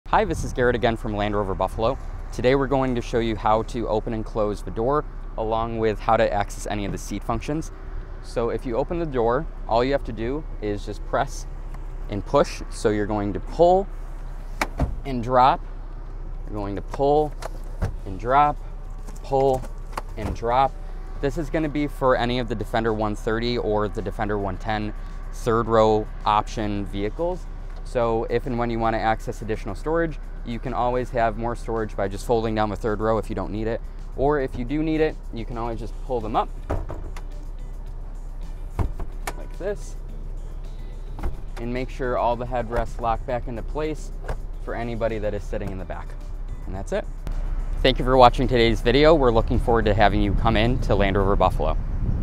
How to Fold Down 3rd Row Seating in Your 2023 Land Rover
This videos explains and demonstrates how to fold down 3rd row seating in your 2023 Land Rover.

If you are looking for more How-To-Videos featuring 2023 Land Rover Infotainment displays: